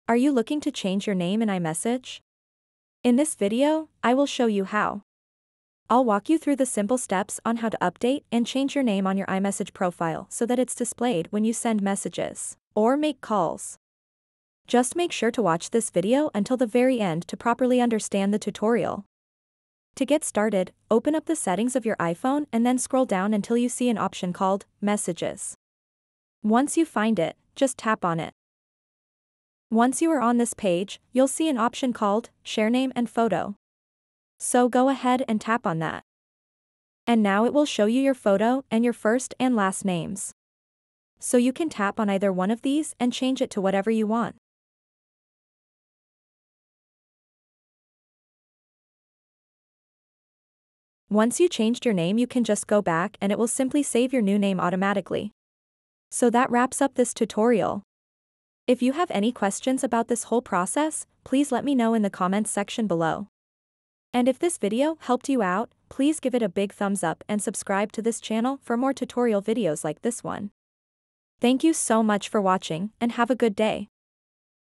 How To Change Your Name In iMessage
Do you want to change your name in iMessage, but don't know how? Then this video is perfect for you! Learn the step-by-step process on how to quickly and easily make the switch. We'll guide you through everything from opening settings, entering your new name, and confirming the changes. So don't wait any longer - watch now and get started with changing up your iMessage profile today!